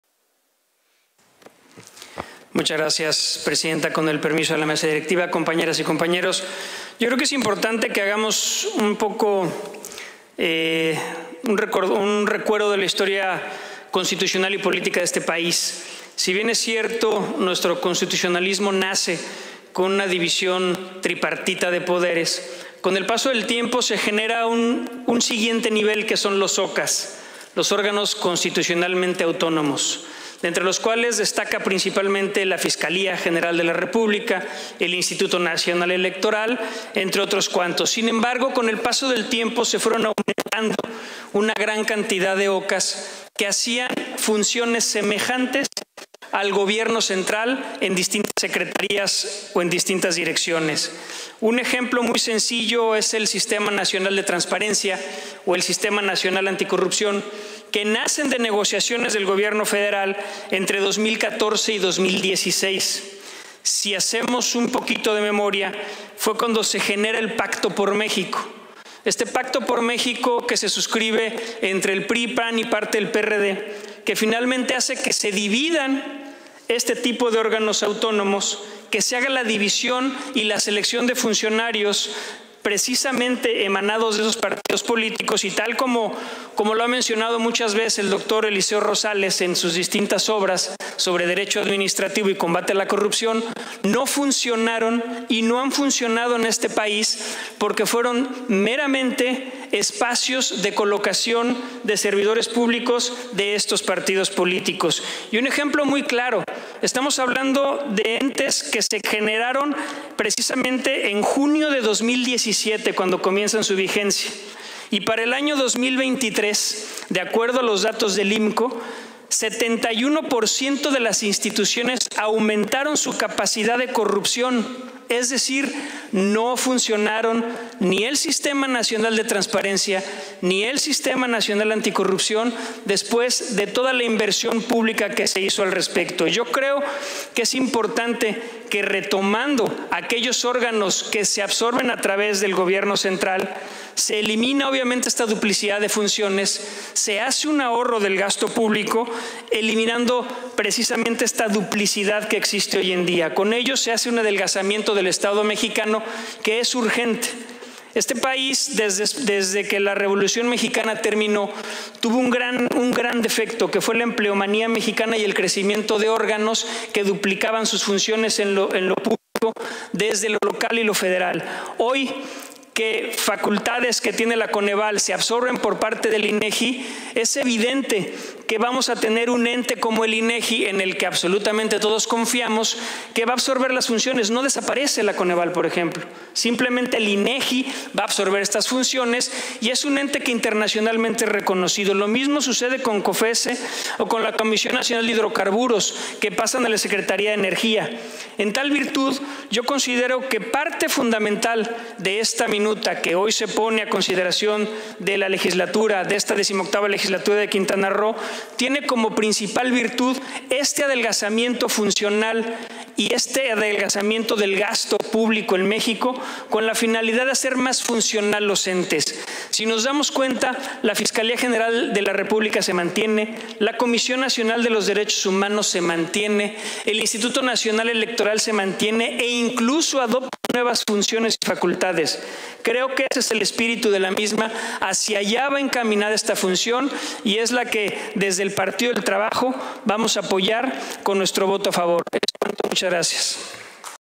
Intervención Dip. Hugo Alday Nieto. PT (04/Noviembre/2024).
Intervención del Dip. Hugo Alday Nieto, del PT en relación a la Minuta Proyecto de Decreto por el que se reforman, adicionan y derogan diversas disposiciones de la Constitución Política de los Estados Unidos Mexicanos, en materia de simplificación orgánica, remitida por la Cámara de Senadores del H. Congreso de la Unión; para su aprobación, en su caso.